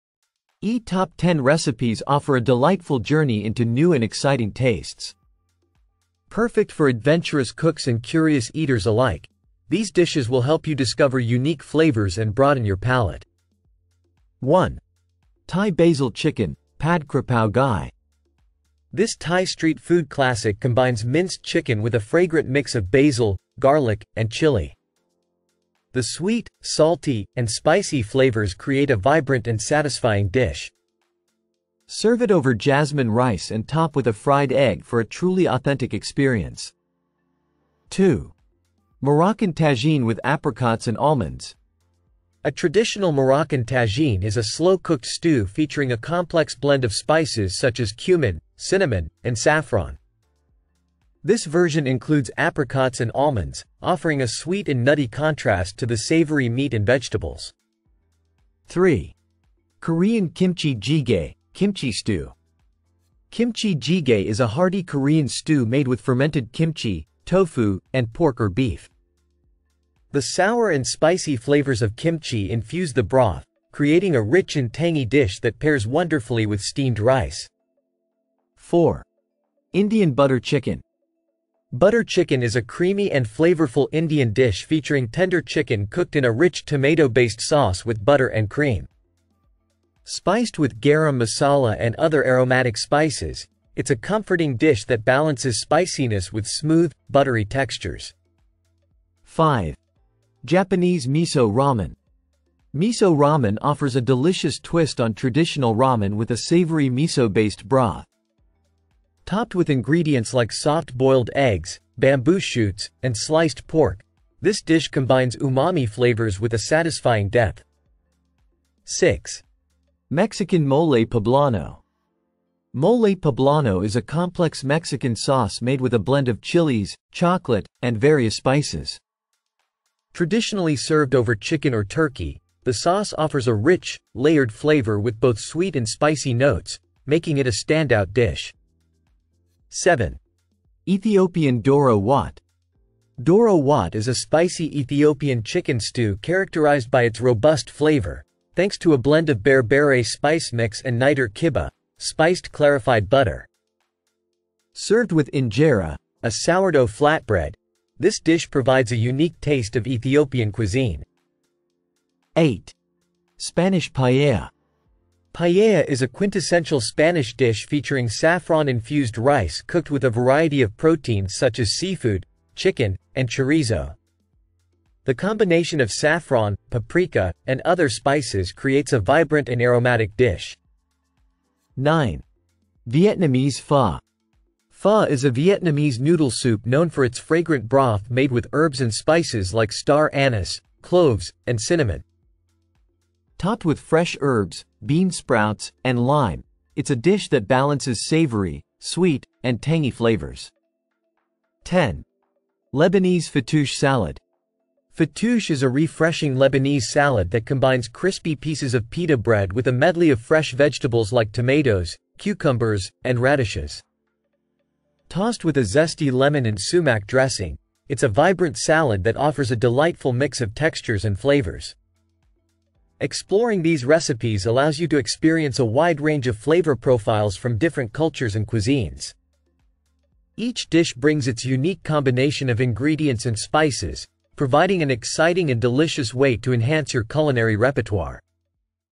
Top 10 Recipes for Exploring New Flavor Profiles
Expand your culinary horizons with the Top 10 recipes for exploring new flavor profiles. Discover innovative dishes that challenge your palate and introduce exciting taste combinations!